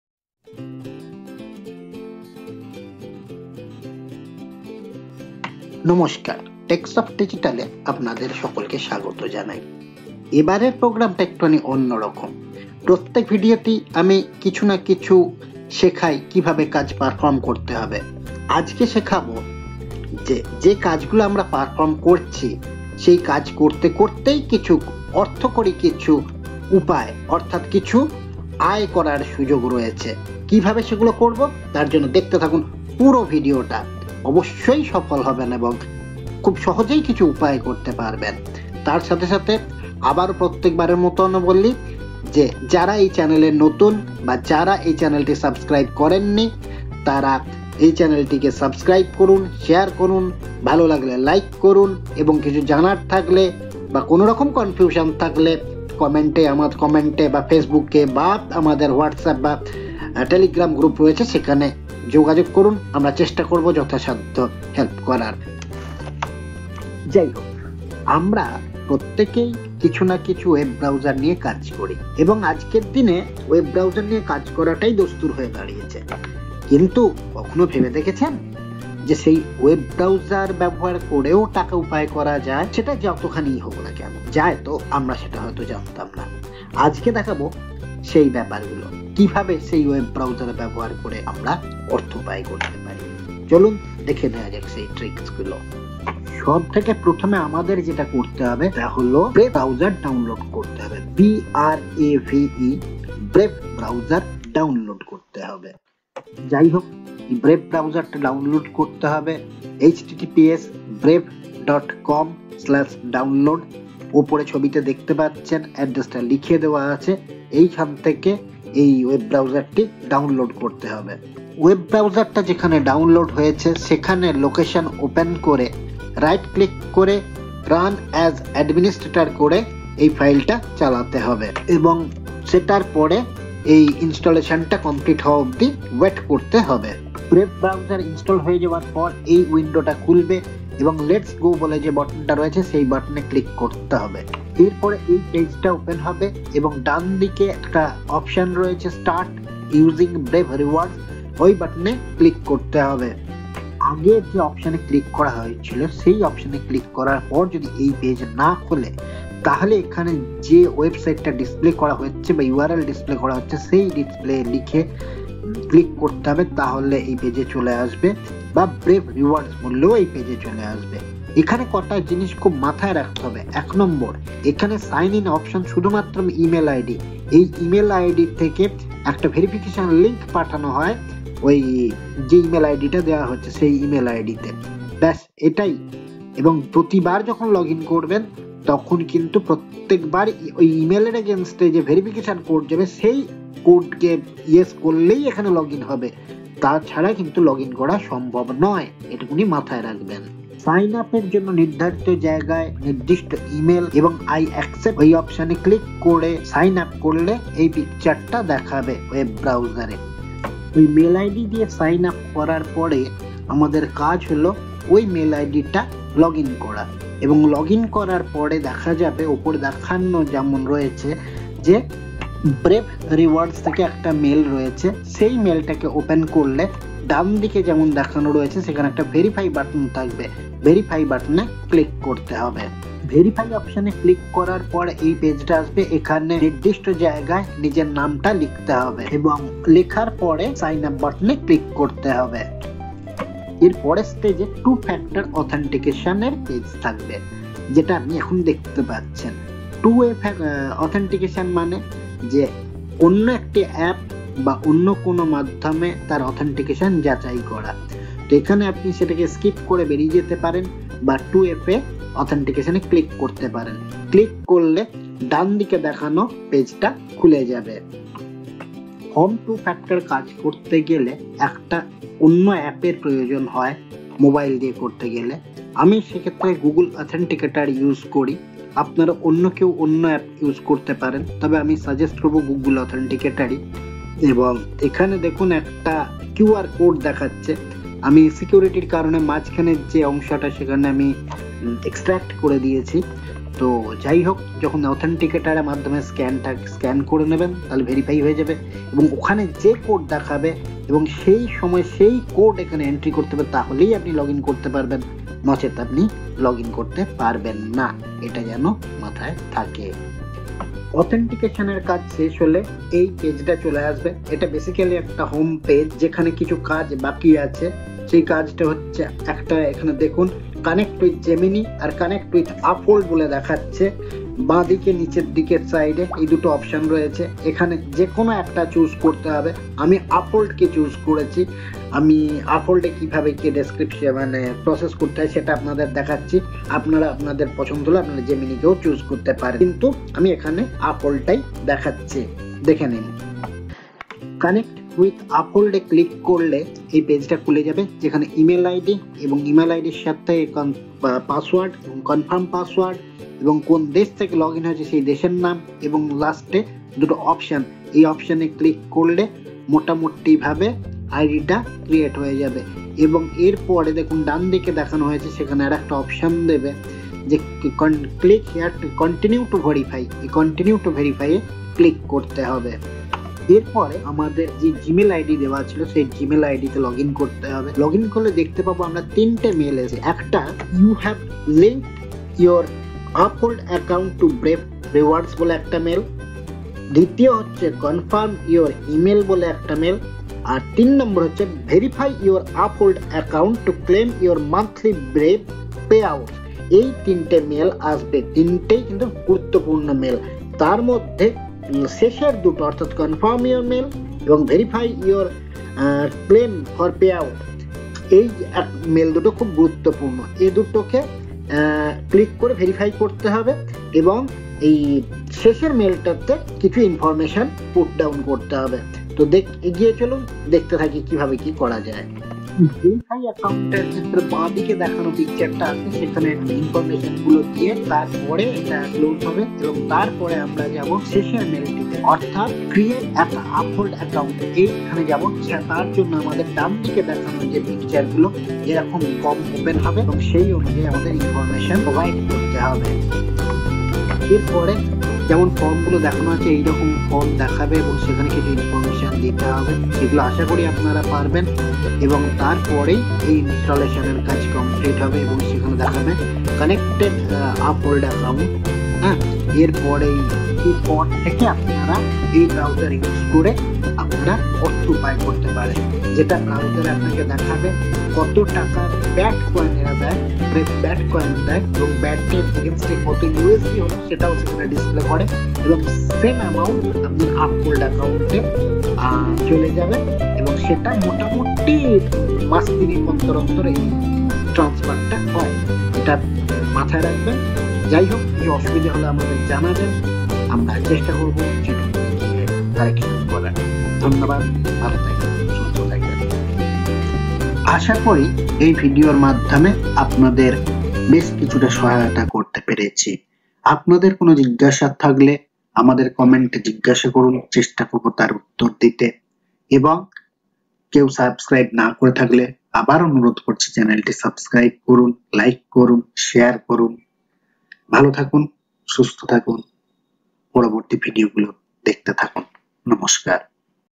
How to Earn Money while you Browse Internet || Online - Use Web Browser To Earn (Bengali)
This video will help you to earn some amount by  replacing your browser by Brave. Which is using google chrome engine.